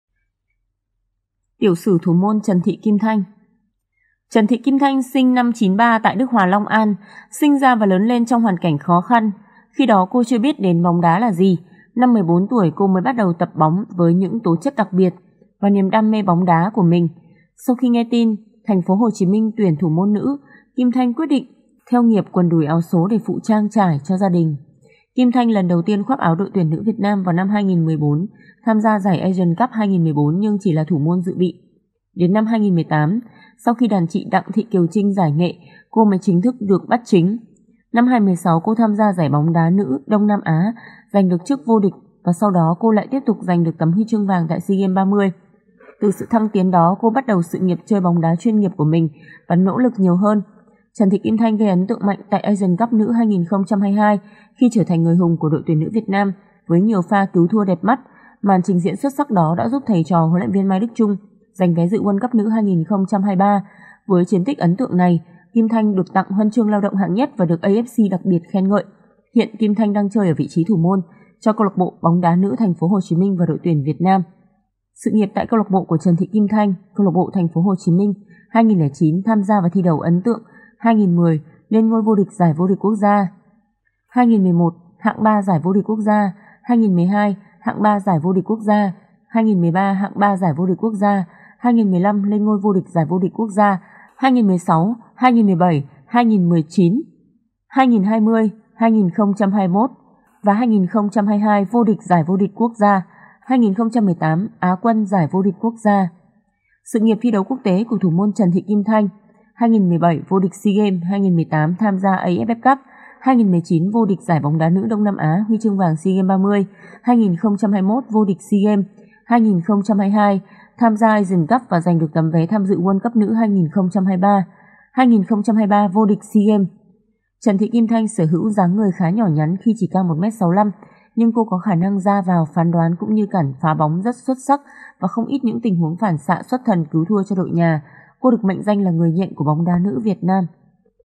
Rơi nước mắt gia cảnh nghèo khó của thủ môn Kim Thanh ĐT nữ VN
Hy vọng chúng tôi sẽ là nguồn kiến thức hữu ích mỗi ngày cho quý vị và các bạn